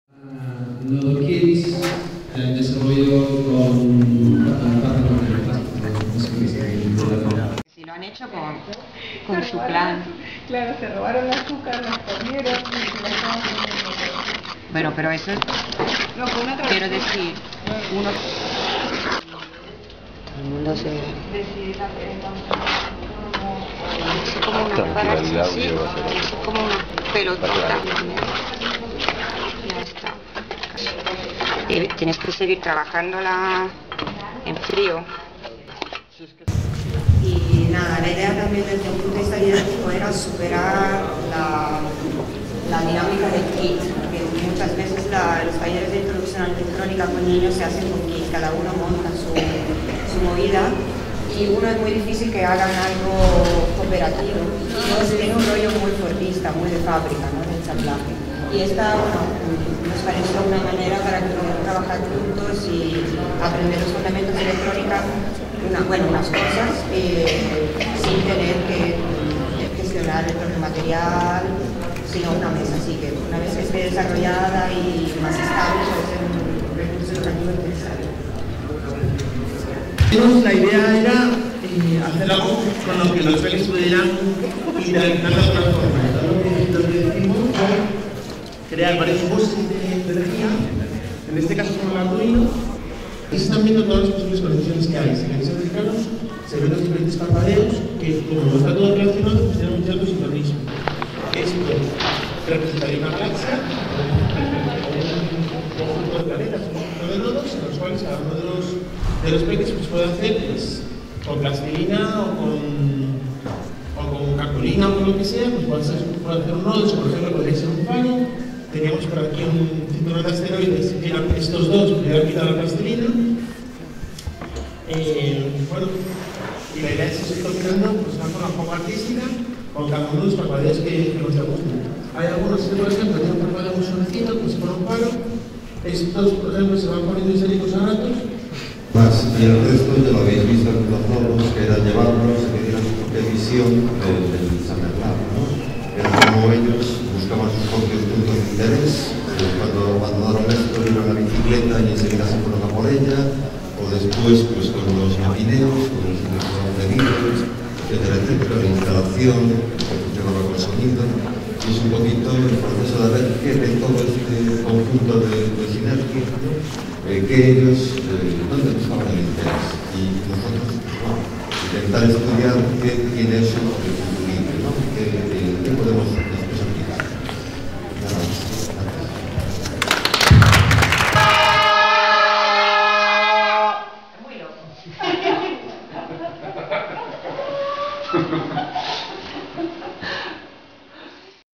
SummerLAB2011-SummerKids.dvd
SummerLAB-2011
LABoral - Gijon

NODO: SummerKids (Susana Tesconi, Miriam Ruiz, Tonecho Otero)

GALAXIA NODAL prototipo de recurso educativo.

Arduino + LEDs + Plastilina Conductiva + Tinta Conductiva (BARE CONDUCTIVE - Beta)

Audio: Binaural
Voices: Pedro Soler (presentacion), Susana Tesconi, Miriam Ruiz, Tonecho Otero)
Edited: Tonecho Otero (Ludidáctica)
Software Open Source: OpenShot Video Editor + GIMP 2.6